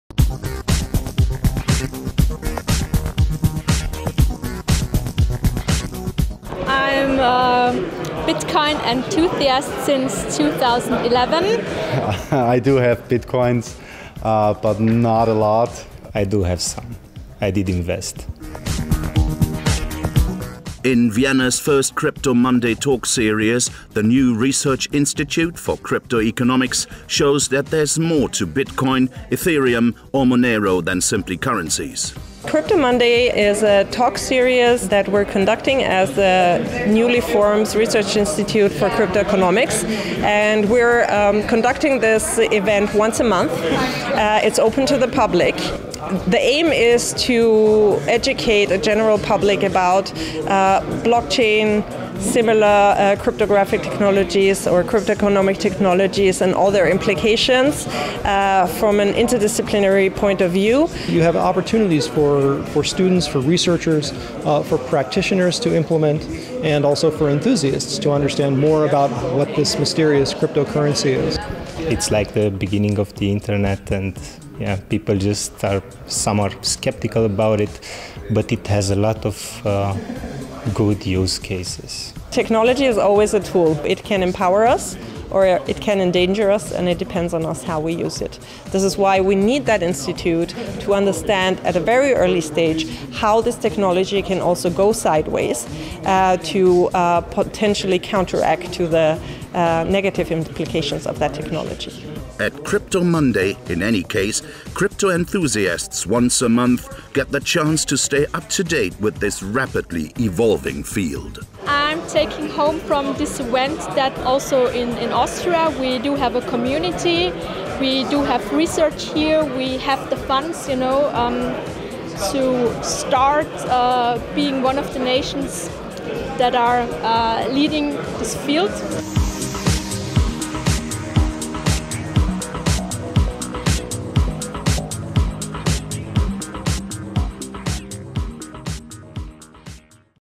The first Crypto Monday at WU Vienna's new founded Research Institute for Cryptoeconomics
Introduction to Cryptoeconomics: What is it and why should I care?